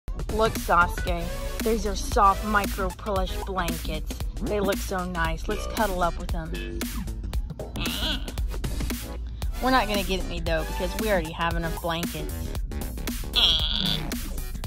Sasuke wants a blanket
My alien caterpillar creature wanted a blanket from Target but I didn't get him one because we already have enough blankets.